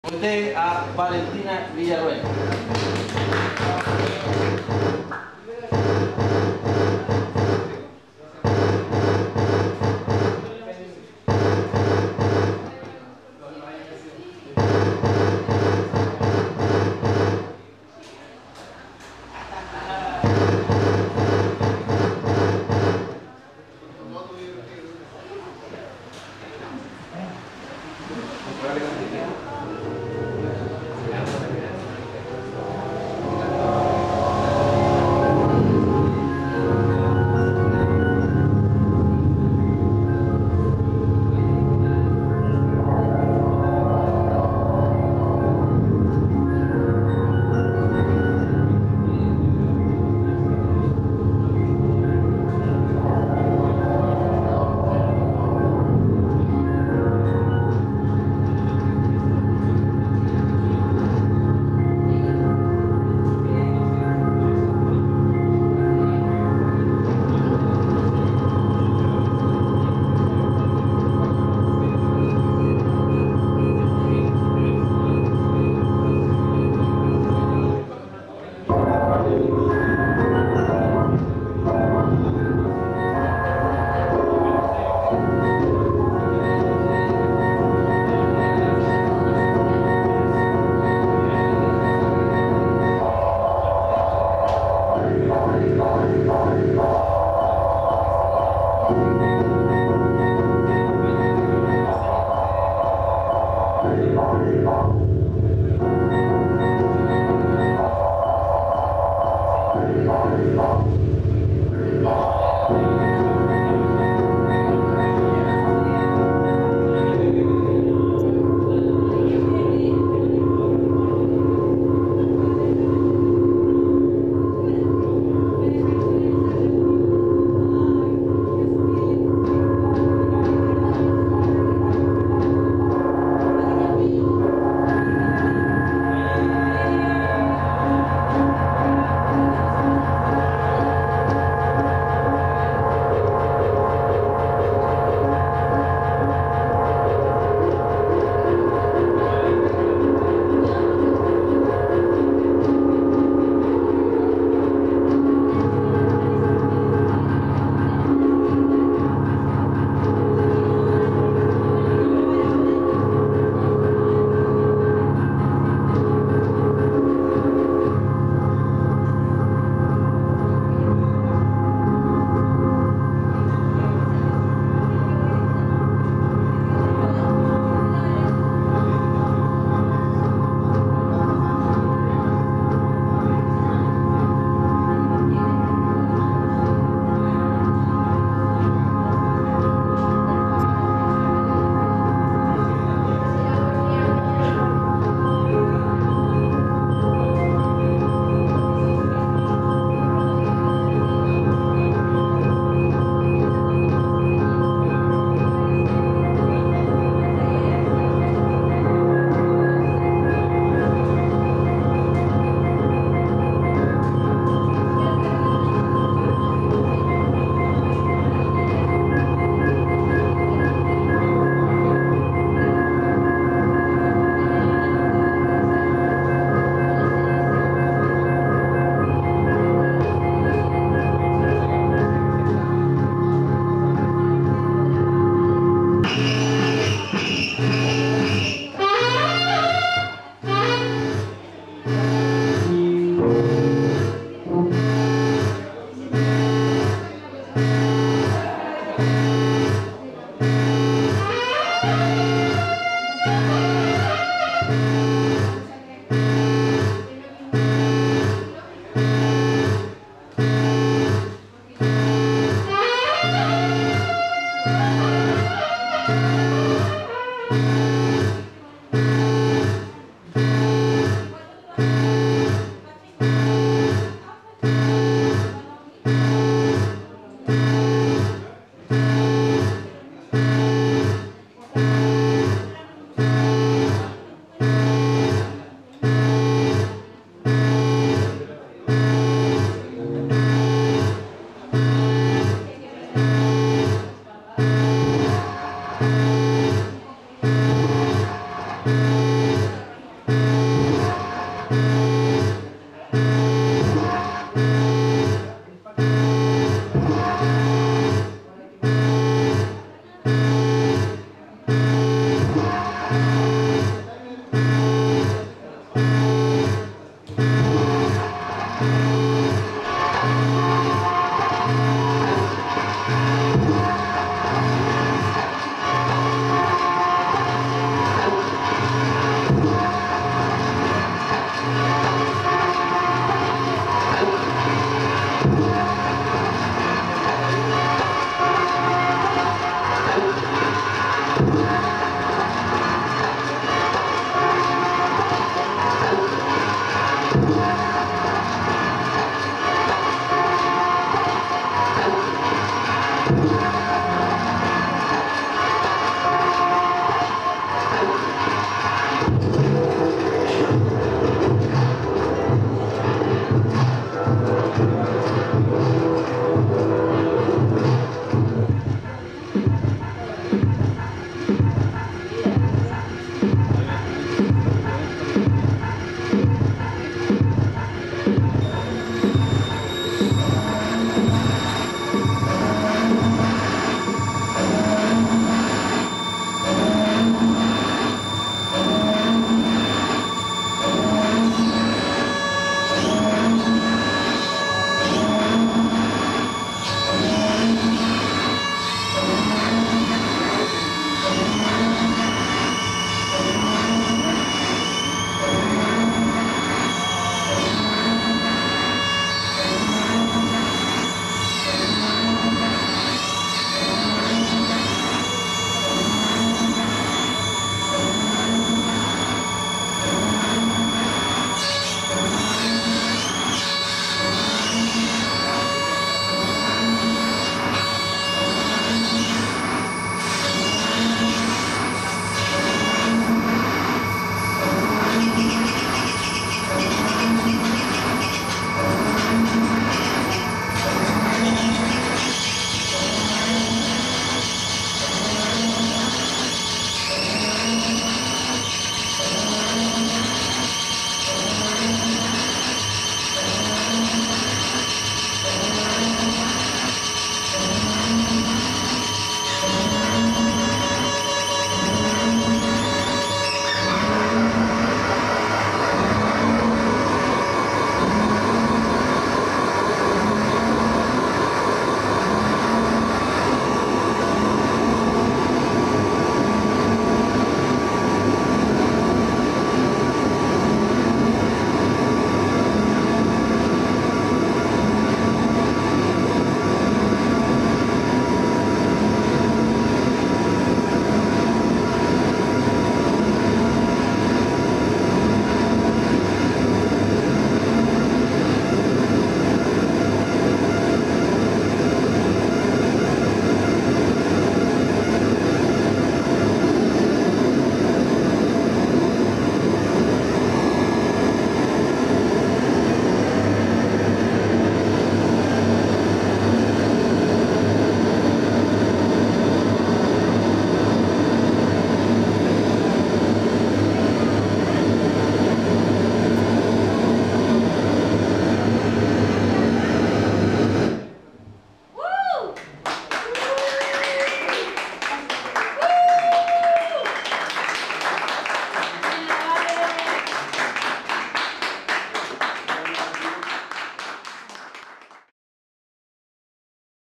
Valentina Villaroel en Singularity Sur (Concepcion,Chile 2014)
Valentina Villaroel en parte de su presentacion en Singularity Sur,en Concepcion Chile